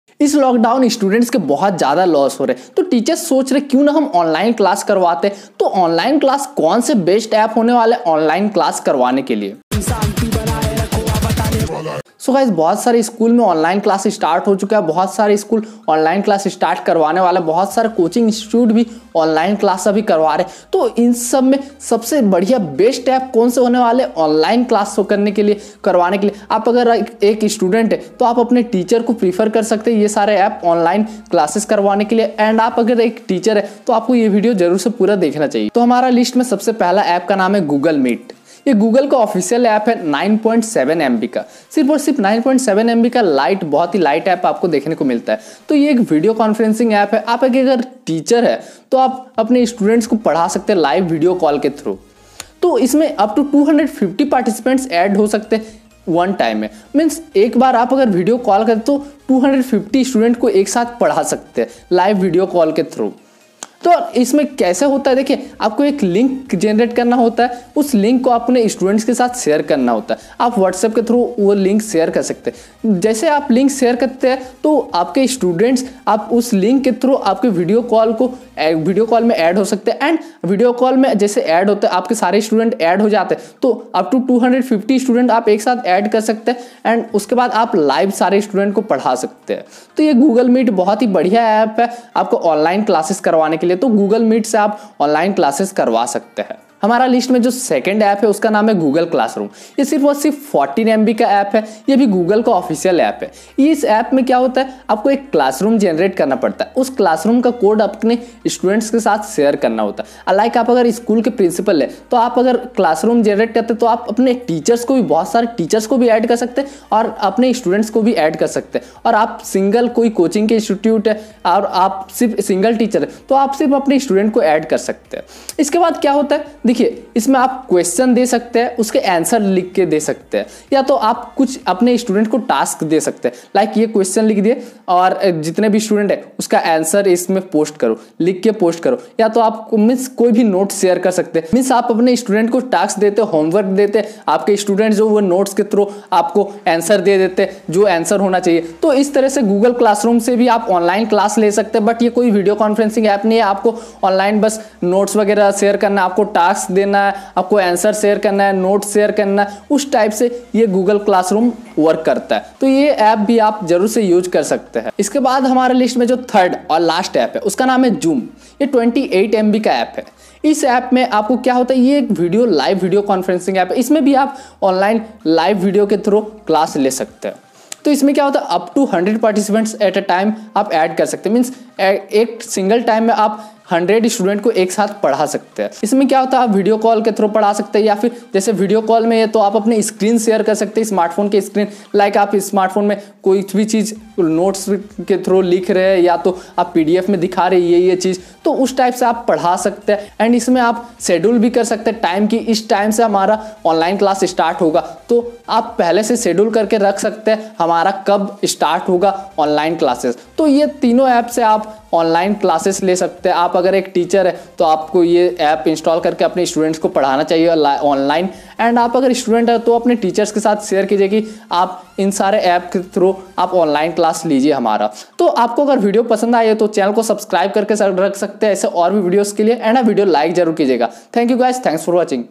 Best Apps for online classes || Top 3 Apps for online class || Aafat Tech
In this video you may know the best apps for online study. Teachers can take online class through this apps